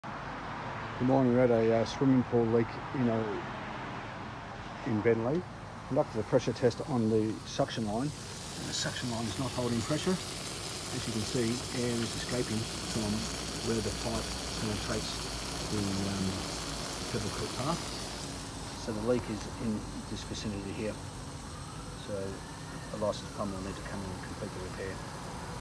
Swimming pool leak detection in Beenleigh
This morning we attended to a reported leaking concrete pool in Beenleigh, we noted wetness on the vacant block beside the pool pump, we noticed bubbles coming from a return jet.
We tested the Muti port valve, before inspecting and dye testing the skimmer box and return jets.
We then entered the pool and inspected and replaced the hydrostatic valve before pressure testing the concealed plumbing which revealed a failed suction line. Air was seen bubbling up from below the pebbelcrete path where the suction pipe connects to the pump.
We talked through our findings with our client who is to engage another contractor to complete the repair.